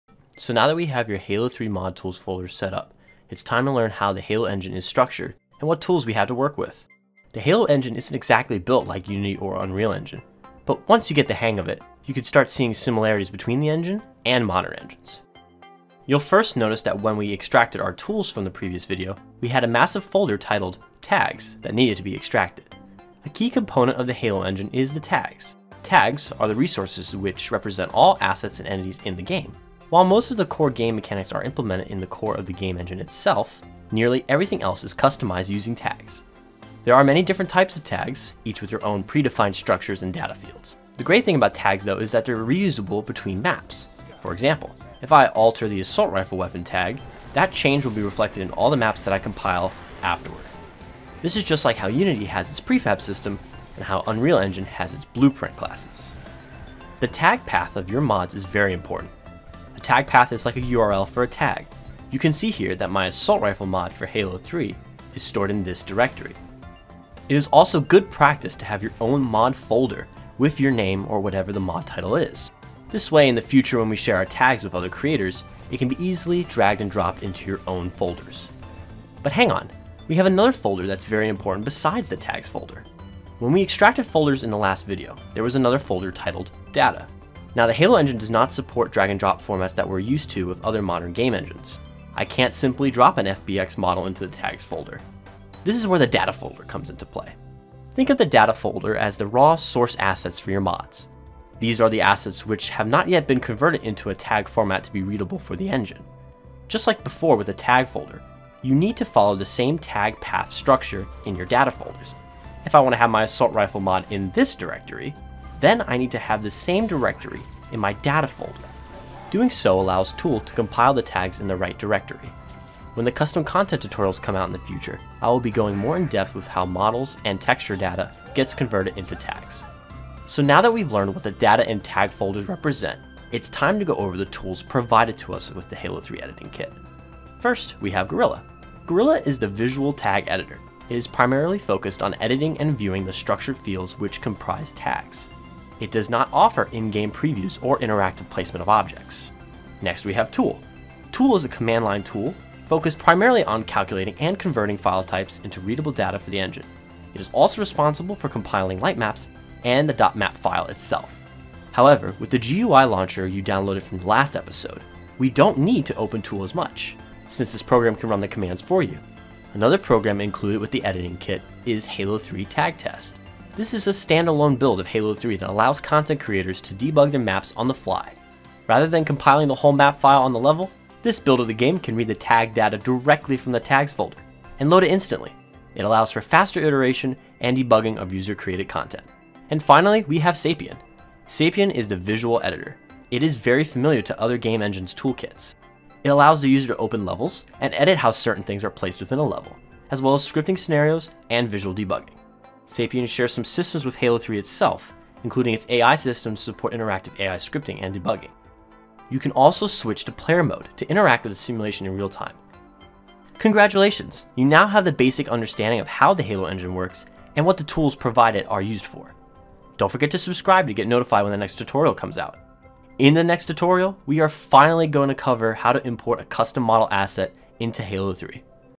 Understanding How Halo 3 Works (Halo 3 MCC Modding Tutorial)
This tutorial goes over the contents of the Halo 3 mod tools and what each program does for the pipeline of Halo 3 content.

Timestamps:
00:00 Intro
00:15 Tags and Data
02:16 Guerilla
02:29 Tool
02:49 halo3_tag_test
03:09 Sapien
03:49 Next Tutorial